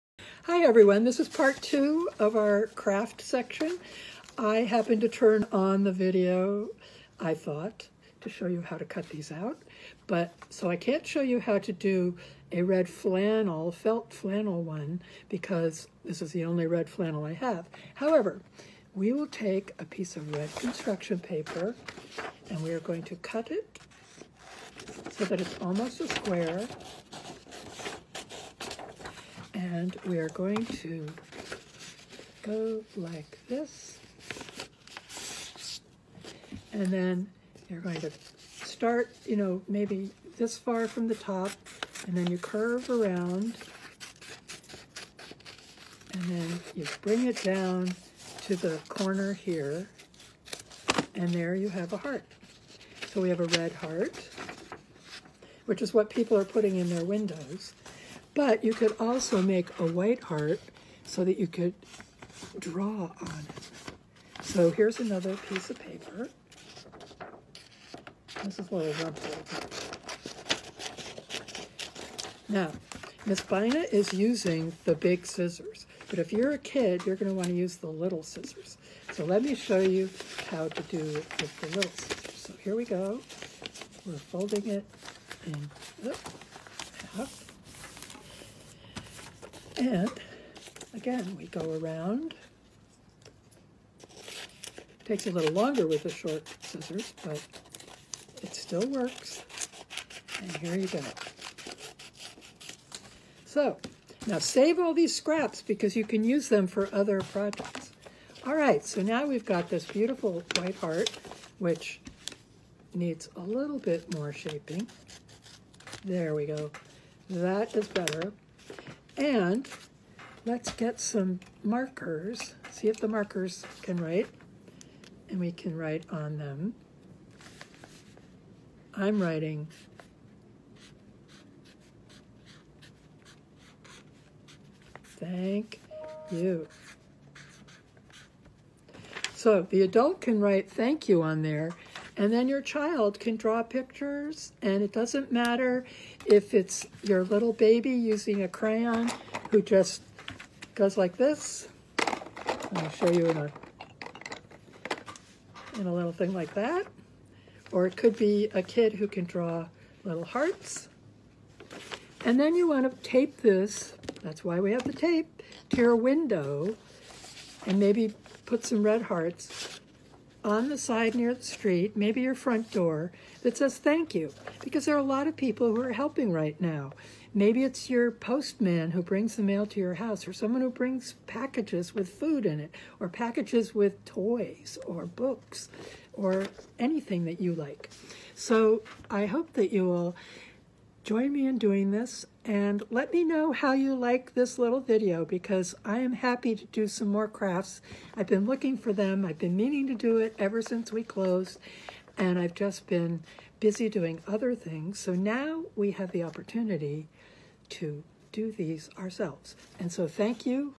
Ms. Bina Craft April 30
It’s LITTLE HANDS time at home!
Let’s make hearts to thank the helpers!
Ms. Bina is a librarian at Bridgeport Public Library.